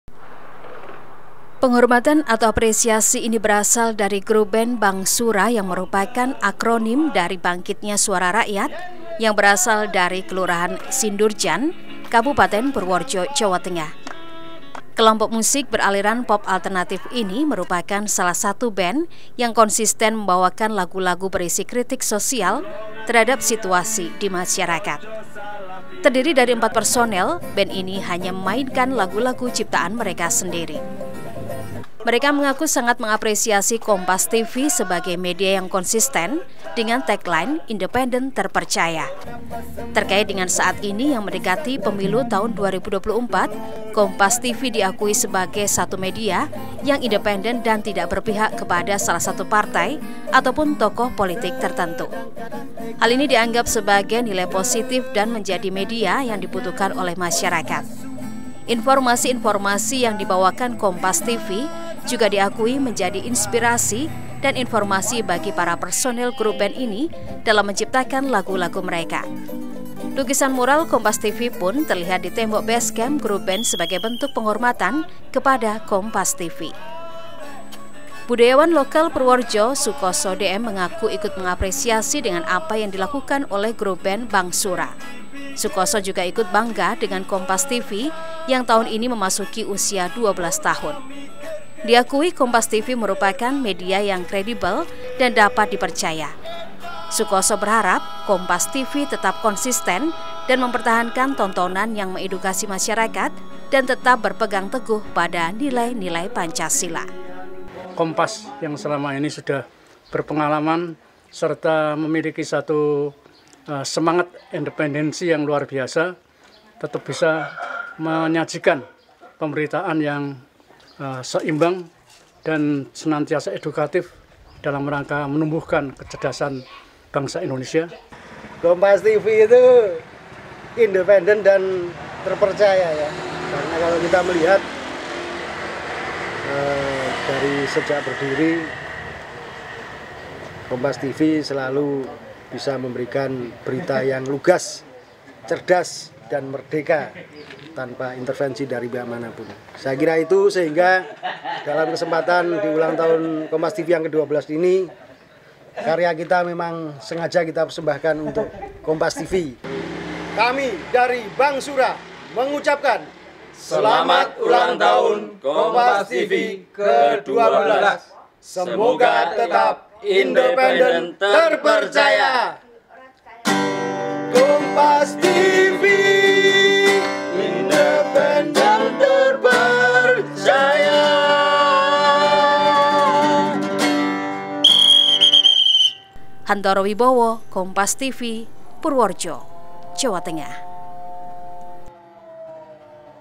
12 Tahun Kompas TV, Grup Band Bang Sura Persembahkan Karyanya
PUROREJO, KOMPAS.TV - Perhormatan atau apresiasi ini berasal dari grup band "Bang Sura" yang merupakan akronim dari bangkitnya suara rakyat yang berasal dari Kelurahan Sindurjan, Kabupaten Purworejo, Jawa Tengah. Kelompok musik beraliran pop alternatif ini merupakan salah satu band yang konsisten membawakan lagu-lagu berisi kritik sosial terhadap situasi di masyarakat. Terdiri dari empat personel, band ini hanya memainkan lagu-lagu ciptaan mereka sendiri.

Mereka mengaku sangat mengapresiasi Kompas TV sebagai media yang konsisten dengan tagline, independen terpercaya. Terkait dengan saat ini yang mendekati Pemilu tahun 2024, Kompas TV diakui sebagai satu media yang independen dan tidak berpihak kepada salah partai ataupun tokoh politik tertentu. Hal ini dianggap sebagai nilai positif dan menjadi media yang dibutuhkan oleh masyarakat. Informasi-informasi yang dibawakan Kompas TV juga diakui menjadi inspirasi dan informasi bagi para personel grup band ini dalam menciptakan lagu-lagu mereka. Lukisan mural Kompas TV pun terlihat di tembok basecamp grup band sebagai bentuk penghormatan kepada Kompas TV.

Budayawan lokal Purworejo, Soekoso DM mengaku, ikut mengapresiasi dengan apa yang dilakukan oleh grup band Bang Sura. Soekoso juga ikut bangga dengan Kompas TV yang tahun ini memasuki usia 12 tahun. Diakui Kompas TV merupakan media yang kredibel dan dapat dipercaya. Soekoso berharap Kompas TV tetap konsisten dan mempertahankan tontonan yang mengedukasi masyarakat dan tetap berpegang teguh pada nilai-nilai Pancasila.

"Kompas yang selama ini sudah berpengalaman, serta memiliki semangat independensi yang luar biasa, tetap bisa menyajikan pemberitaan yang seimbang dan senantiasa edukatif dalam rangka menumbuhkan kecerdasan bangsa Indonesia," tutur Soekoso.

"Kompas TV itu, independent dan terpercaya ya, karena kalau kita liat dari sejak berdiri Kompas TV selalu bisa memberikan berita yang lugas, cerdas, dan merdeka tanpa intevensi dari manapun. Saya kira itu, sehingga dalam kesempatan ulang tahun Kompas TV yang ke 12 ini karya kita sengaja kita persembahkan untuk Kompas TV," ucap Japar, personel Bang Sura